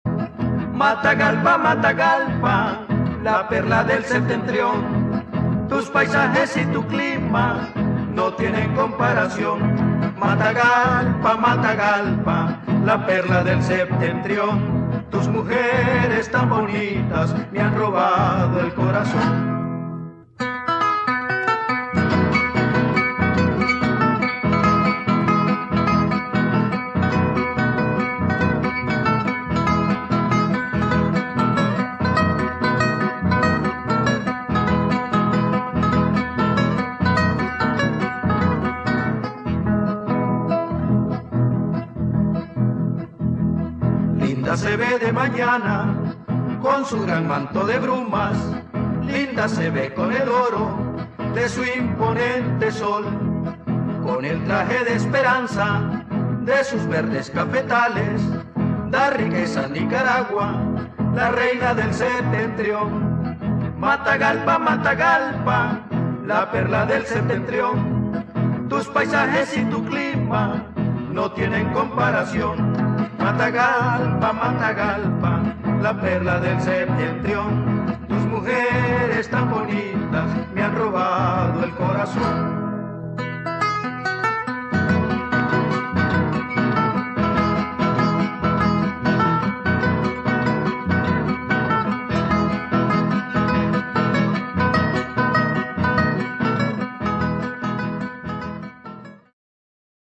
Matagalpa, Nicaragua
Matagalpa ciudad donde el clima fresco de montaña te da la bienvenida, tierra de café, cuyo nombre significa «Casa de las redes o caserío enredado», de origen nahuatl, matla- cal-pa.

Matagalpa cabecera del departamento de Matagalpa situado en el centro del país. Es la séptima ciudad más grande de Nicaragua, la más grande del interior del país y una de las más activas comercialmente fuera de Managua. Es conocida como «Perla del Septentrión», debido a sus características naturales y como la «Capital de la Producción», por su variada actividad agropecuaria y comercial. Está ubicada a 128 kilómetros al noreste de Managua, la capital del país.